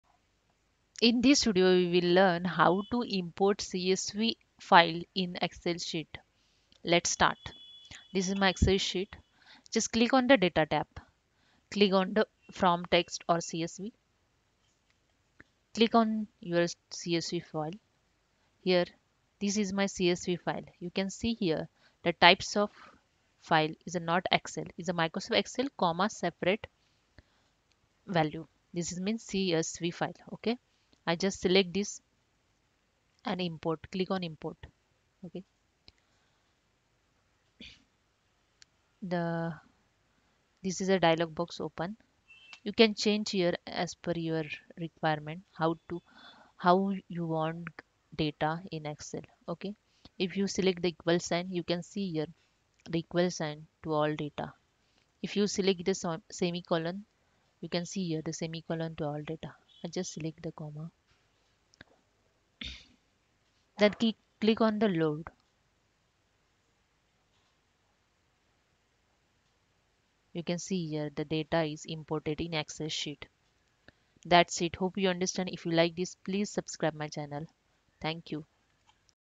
How To Import Csv Data In Excel Sheet
Open Excel.
Click New, then click on Blank workbook.
Click on the Data tab.
Click Get External Data From Text.
Navigate to the CSV-formatted file saved on your system, select it, then click Import (When browsing to find the file, set browser to look for All Files or for Text Files)